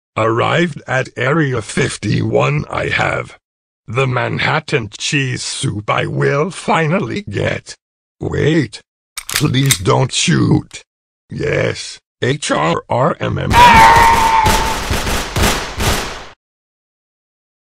[ASMR] Yoda Attends the Area 51 Raid
Yoda does indeed attend this raid and the results may shock you.